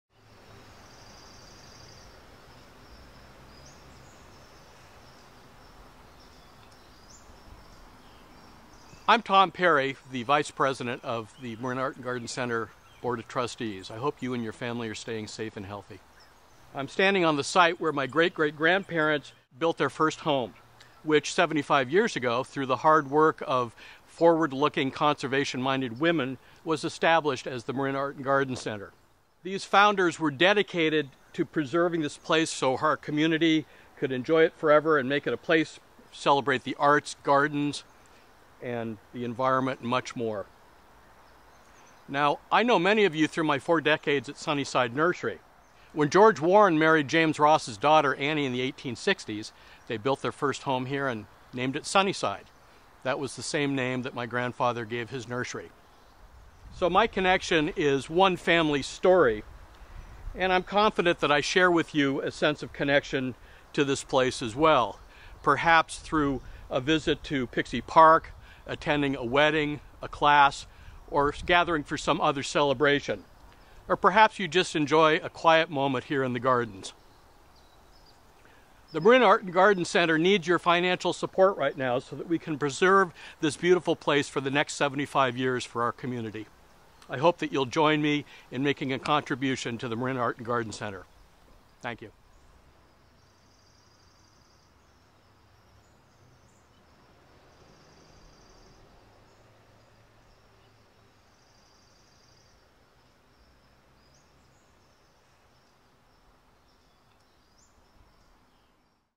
Tom Perry's message of support
Tom Perry, Vice President of the Board of Trustees and a descendant of the property's early owners, encourages you to support Marin Art and Garden Center, a true community resource for generations.